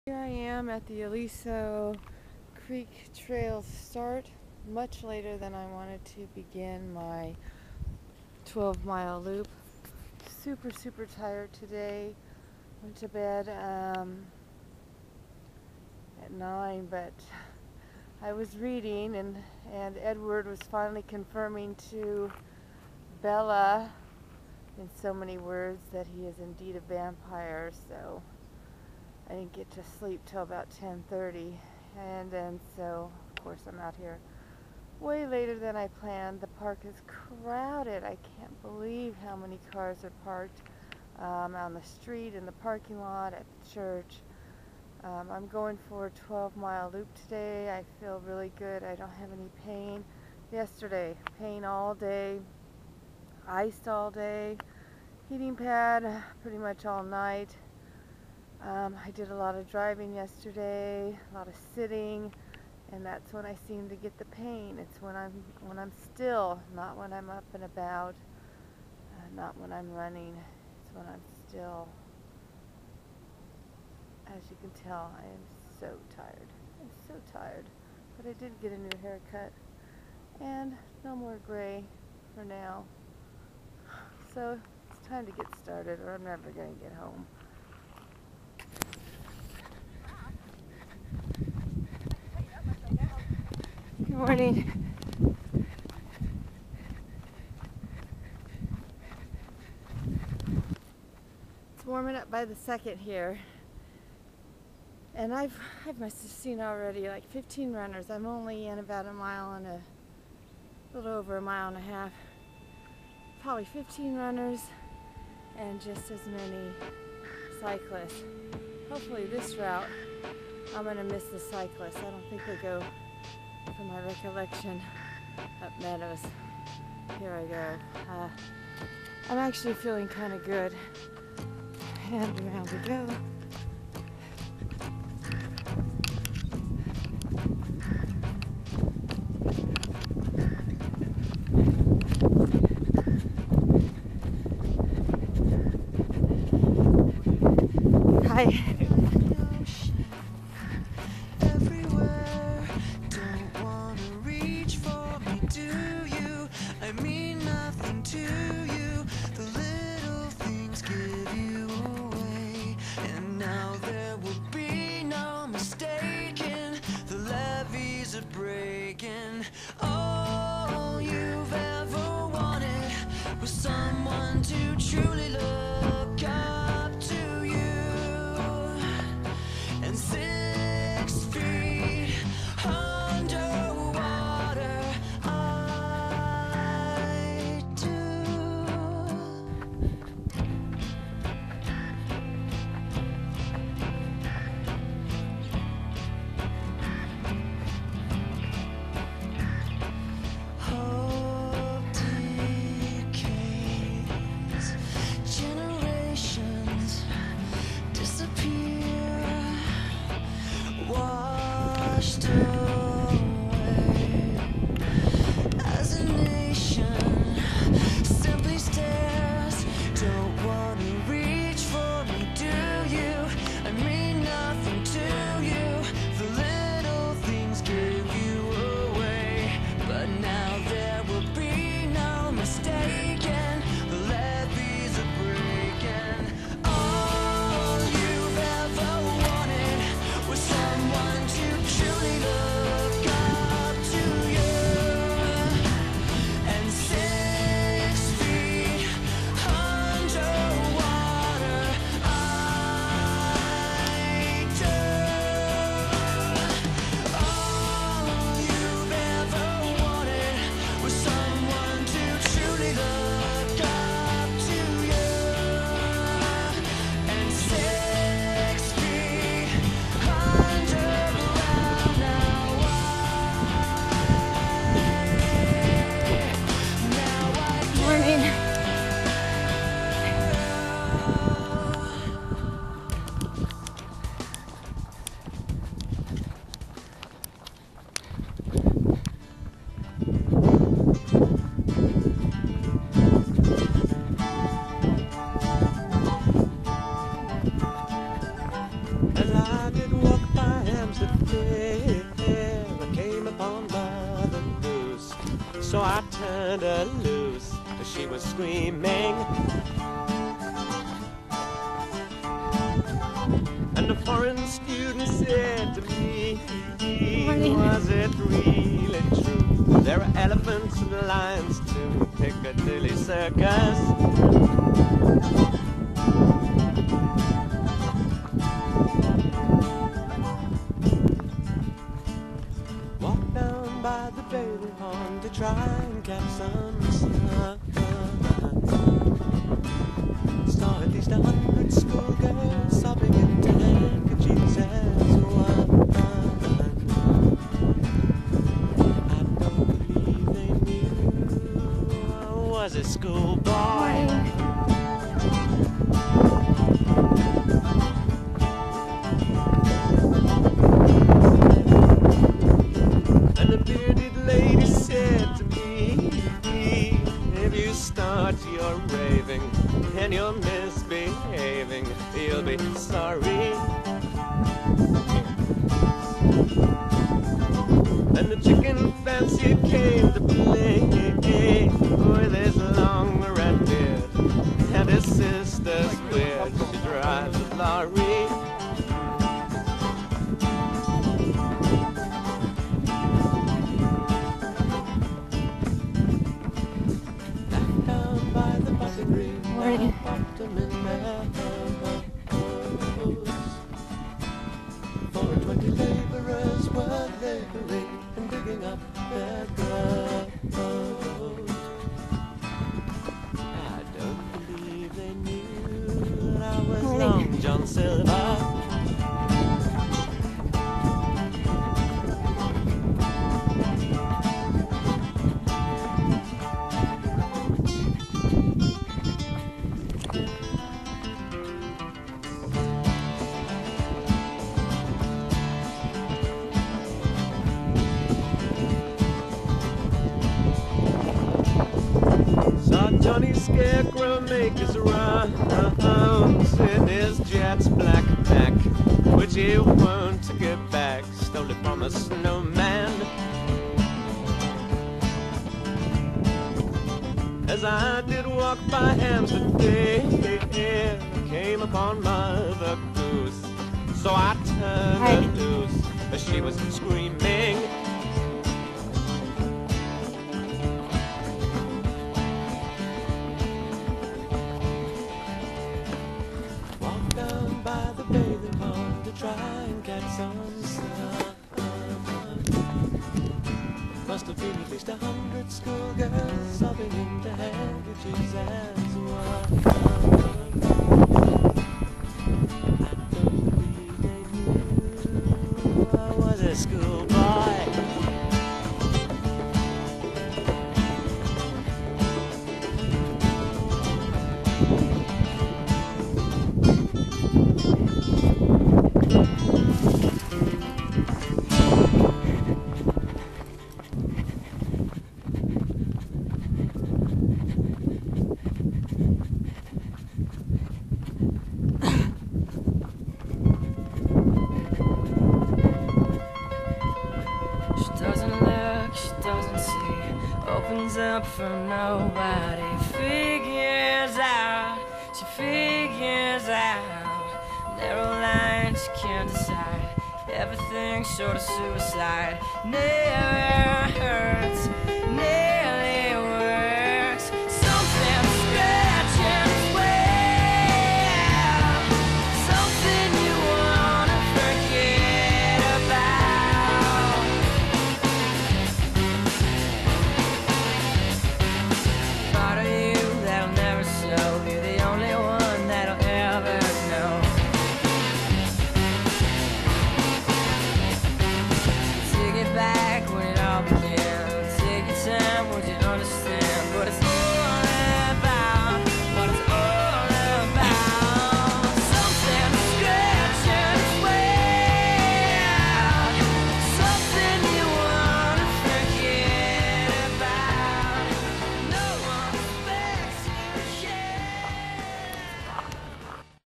The Big Loop / Aliso Wood Canyons
Taking the outer edge trails clockwise, Aliso Creek, to Wood for a few steps, up Meadows, across Top of the World and Westridge, down Cholla to Wood Cyn, to finally end where I started, Aliso Creek Trail